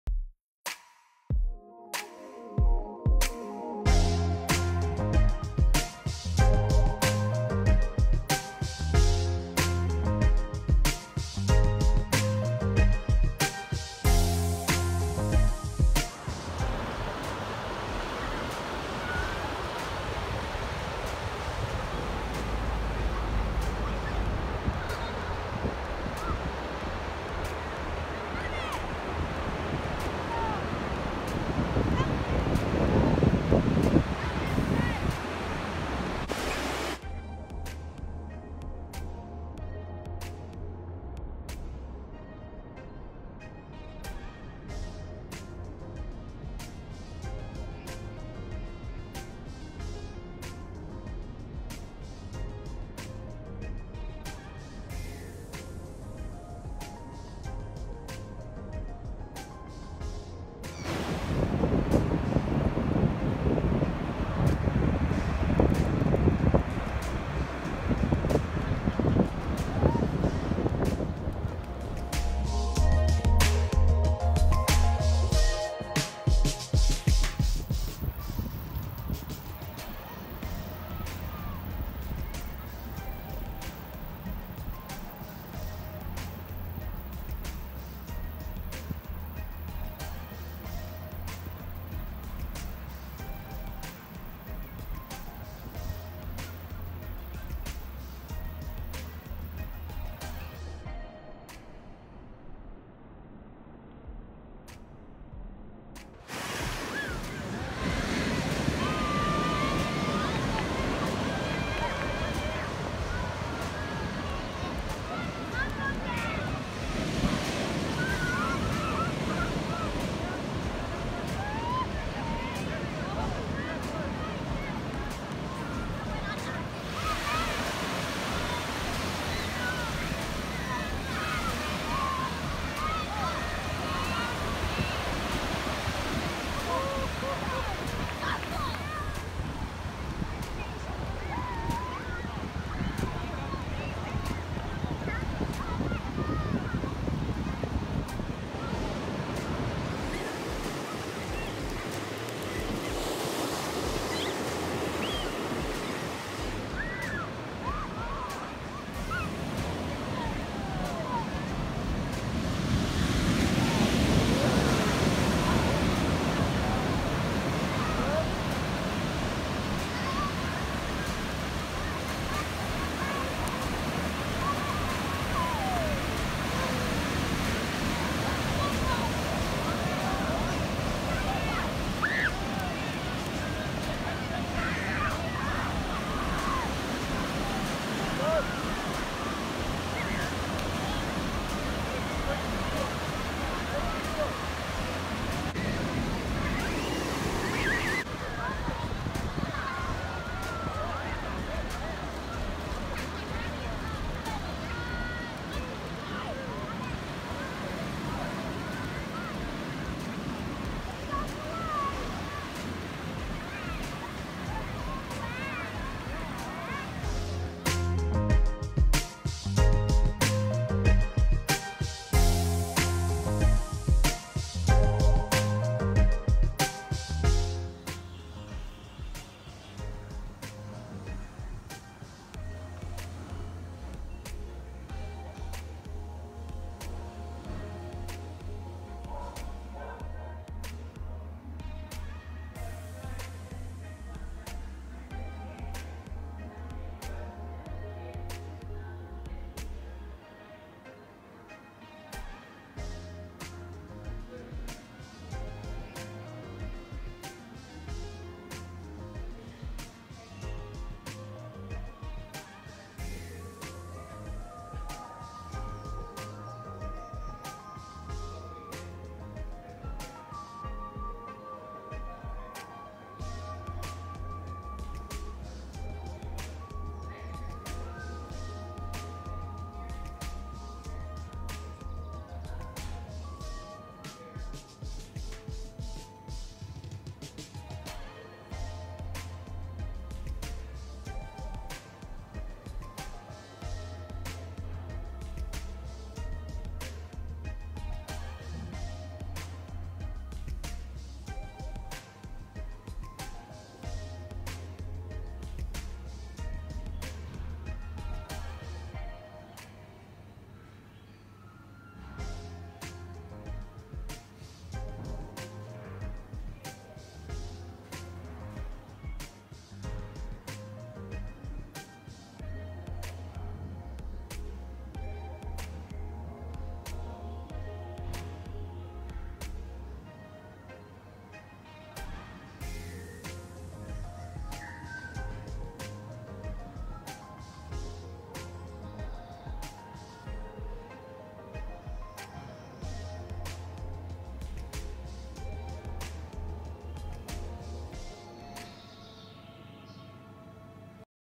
Castlepoint New Zealand
Just 50 minutes from Masterton, is famous in New Zealand for its lighthouse,safe swimming,fishing and surfing 🏄‍♀️